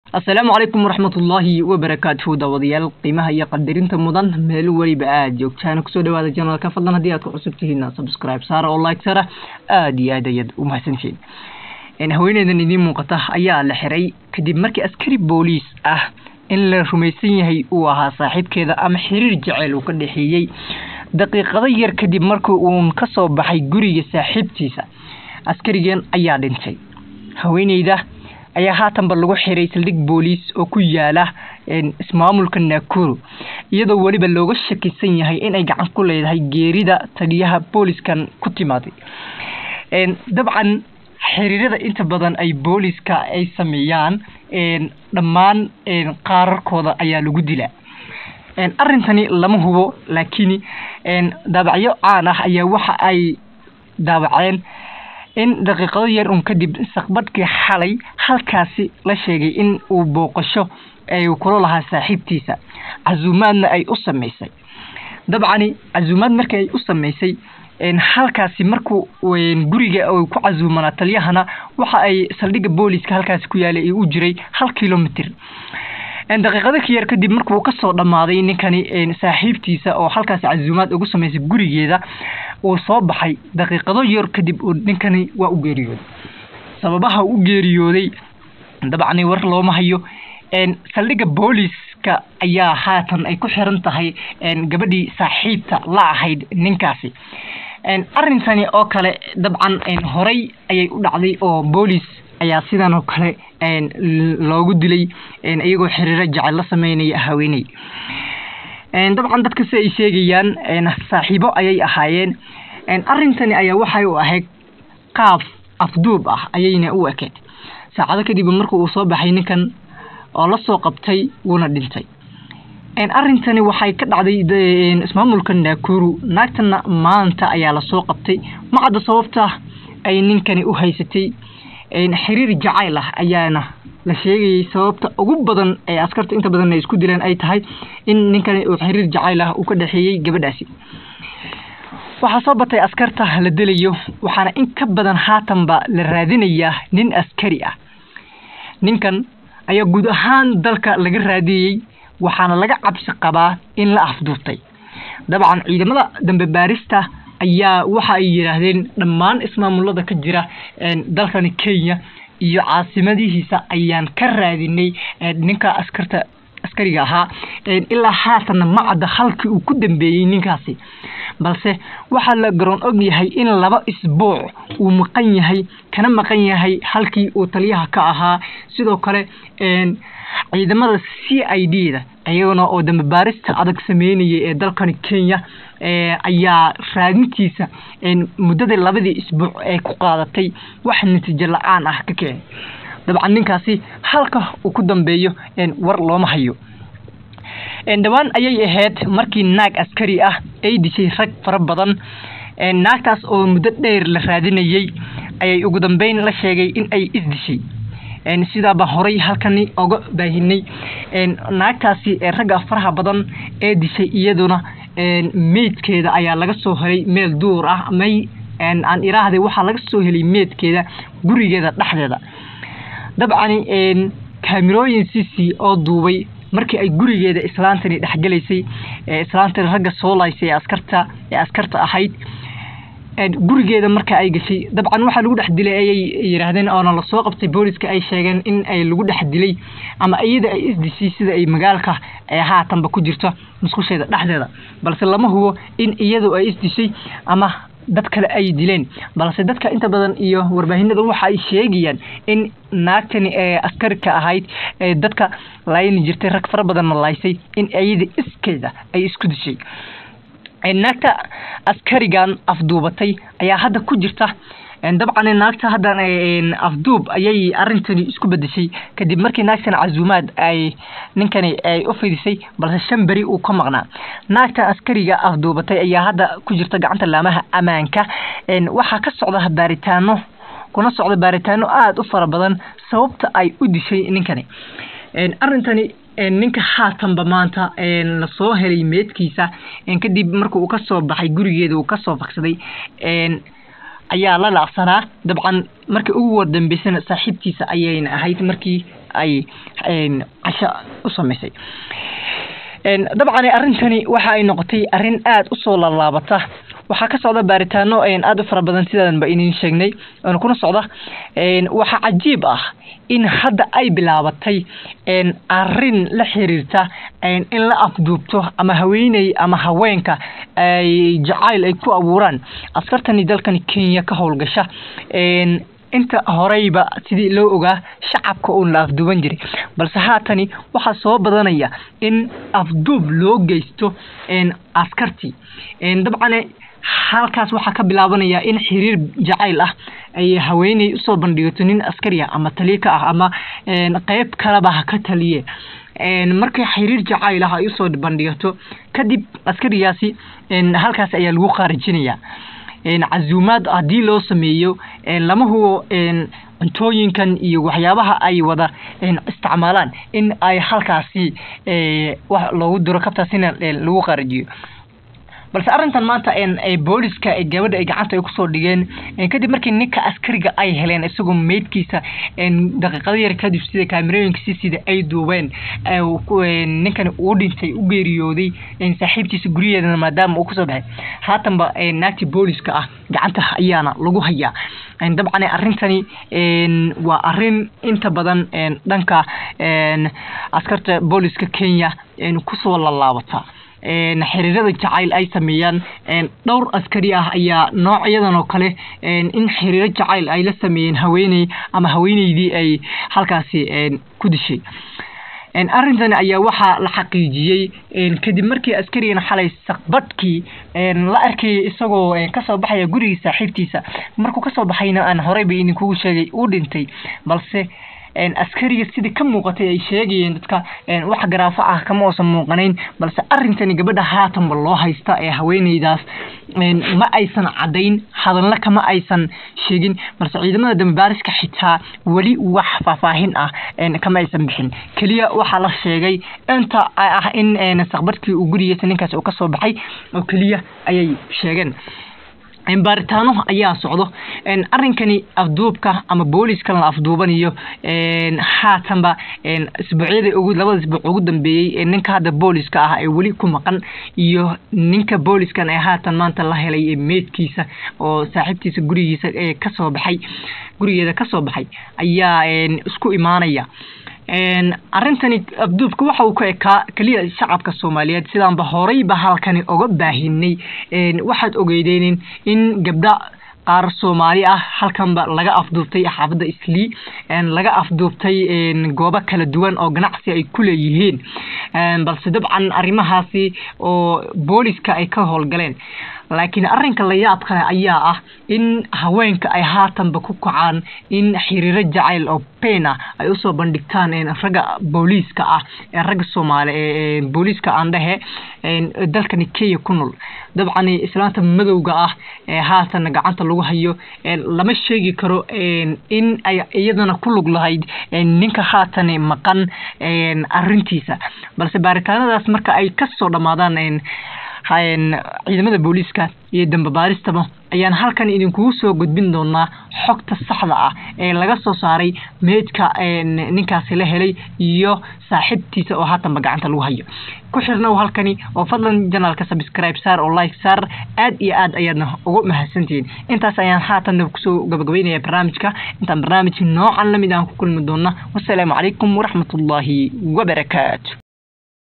DAAWO Naag nin askari ah afduubatay kadibna dishay iyo habka looso qabtay maanta
Cajiib